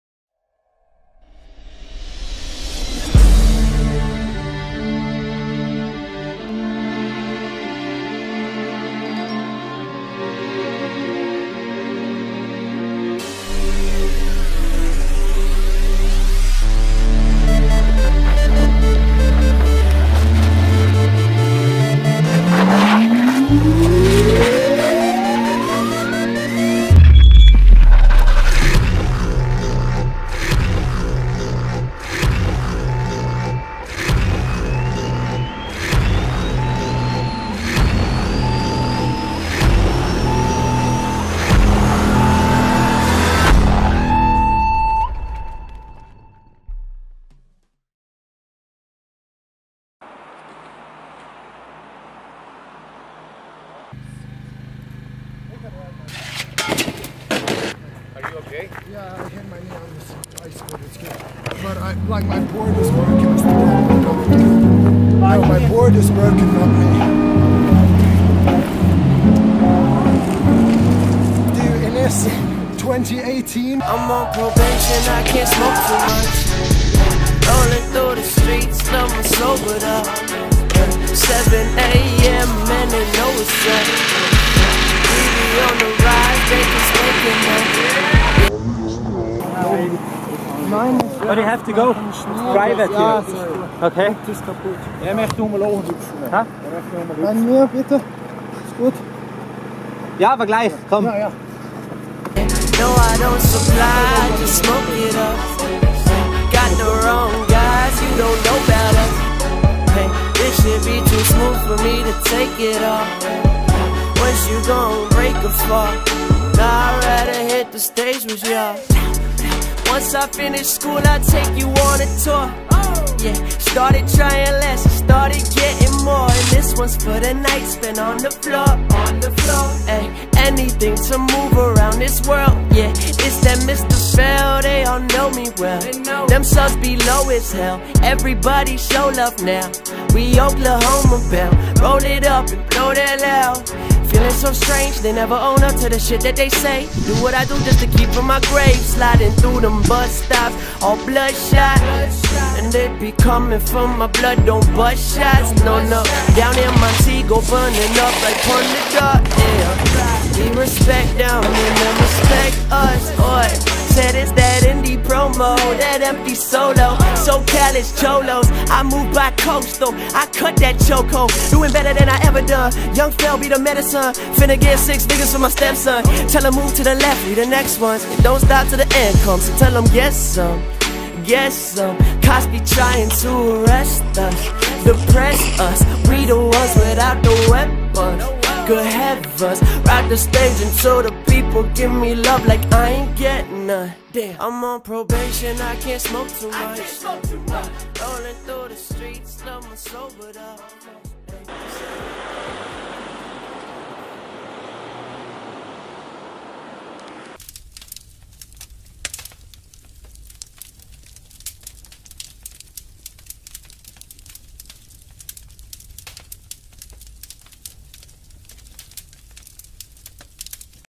RUINA// Full Part// Stuart Innes//2017-18// Street Snowboard Footage
Buddy cam-ing some street snowboard spots around Mayrhofen and Innsbruck area, with Alberto and Pau back during the 16/17 season.

Edit Alberto